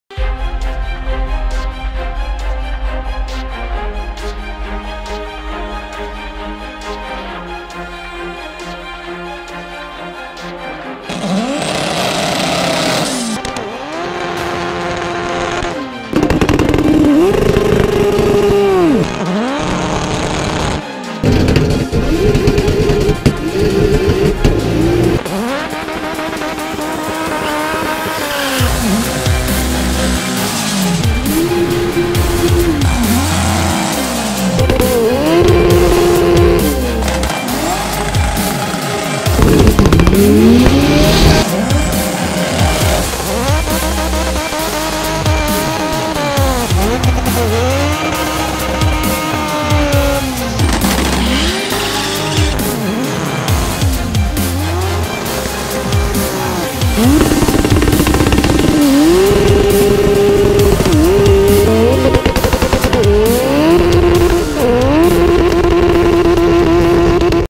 QCFWD 3-4 Sept 2016 Napierville Dragway AdrenalineQC Drag Racing
WINNER 2000$ QCFWD Sept 3-4 2016 Napierville Speedway
Quebec Front Wheel Drive Battle - MD Tuning, BCF, SplitFire & more!!
Confirmed Racer: JF Kelly Fastest Civic in Canada @ 8.73
Facebook: AdrenalineQC Drag Racing ~ Adrenaline QC